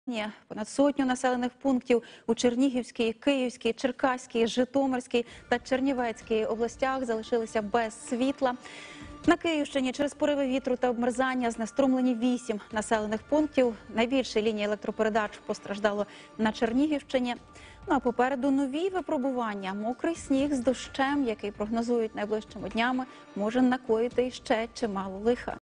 Понад сотні населених пунктів без електрики: наслідки негоди
У зв’язку зі складними погодними умовами без електрики залишилися 107 населених пунктів у 5 областях України.